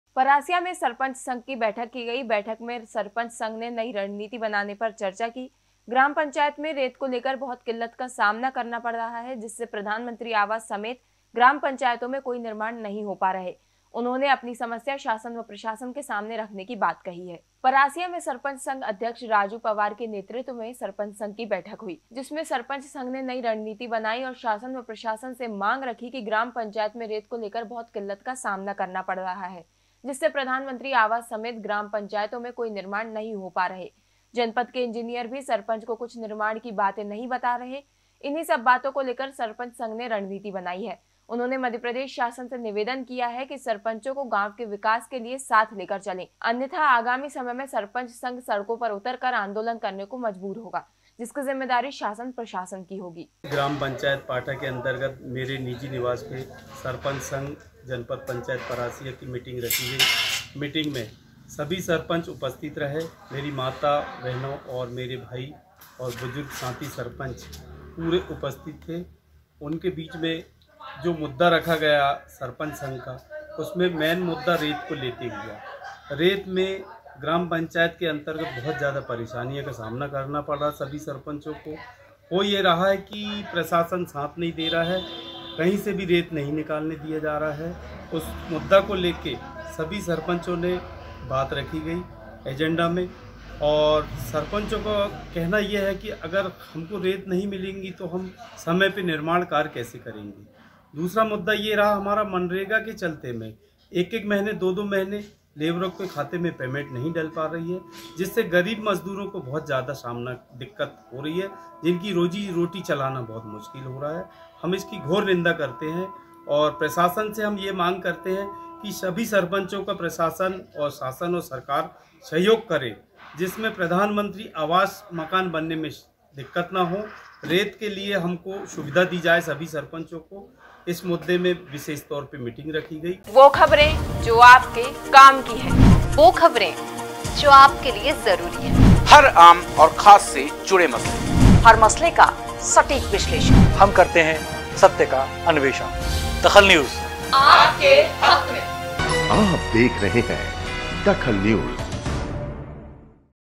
DAKHAL NEWS सरपंच संघ अध्यक्ष के नेतृत्व में हुई बैठक
सरपंच संघ अध्यक्ष के नेतृत्व में हुई बैठक
प्रशासन के सामने रखी अपनी समस्या